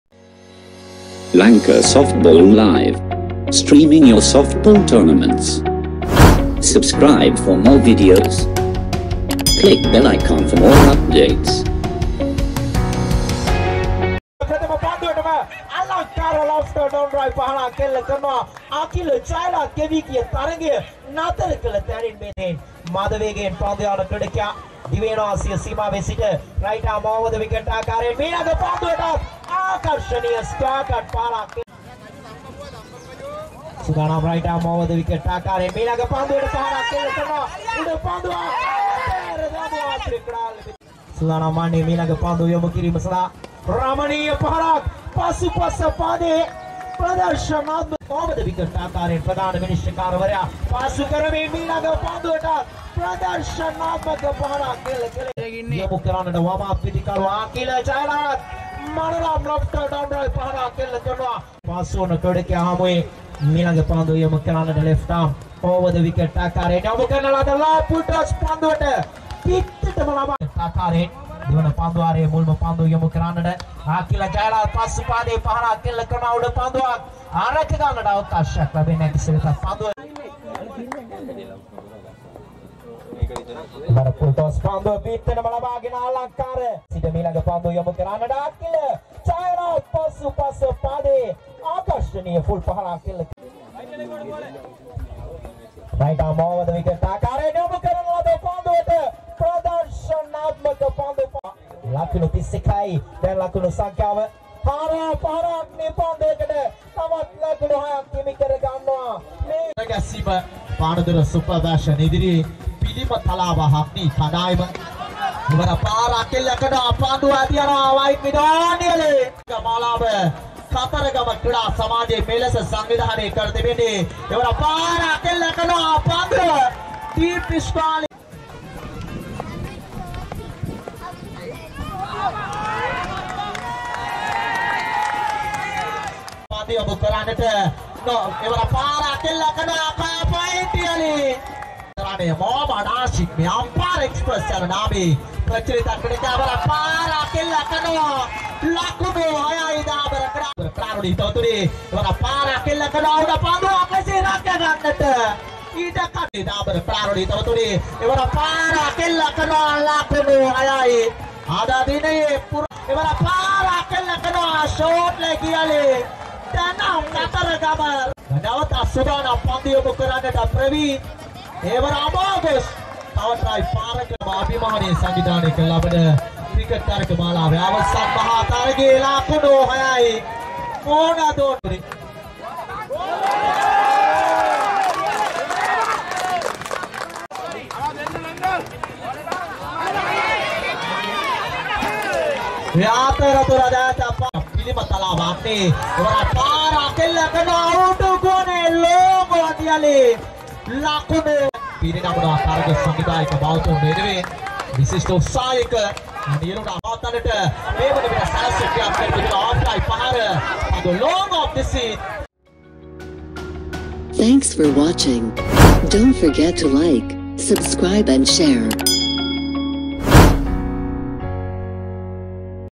බැටින් නම් බැටින්😍🏏💪 SUPER BATTING PERFORMANCE - AKILA JAYALATH (AGNI)
🔴කතරගම ක්‍රීඩා සමාජය සංවිධානය කල දැවැන්තම ක්‍රිකට් තරගාවලිය තුල පිලිමතලාව අග්නි කණ්ඩායමේ වමත් ආරම්භක පිතිකරු
අකිල ජයලත් එල්ල කල සුපිරි පහරවල් සියල්ලම  HD VIDEO